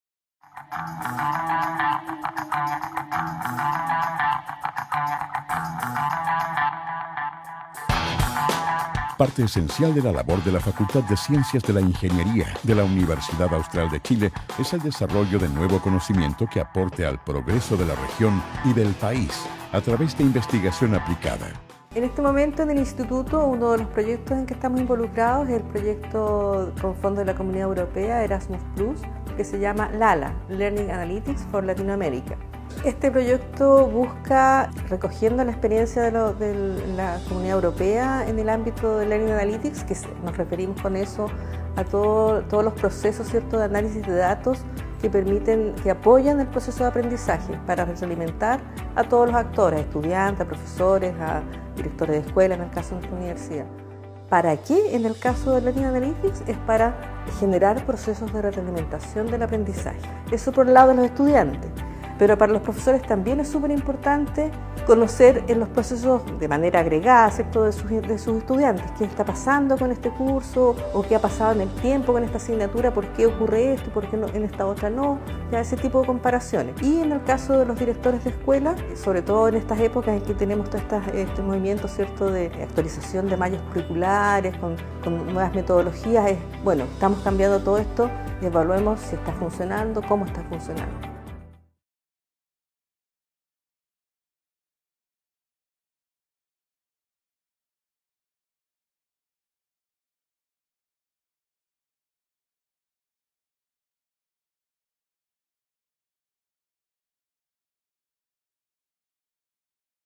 Pʀᴏʏᴇᴄᴛᴏ LALA
Te invitamos a conocer parte del trabajo de investigación desarrollado en el Instituto de Informática UACh de la Facultad de Ciencias de la Ingeniería UACh dentro del marco de desarrollo del Proyecto LALA.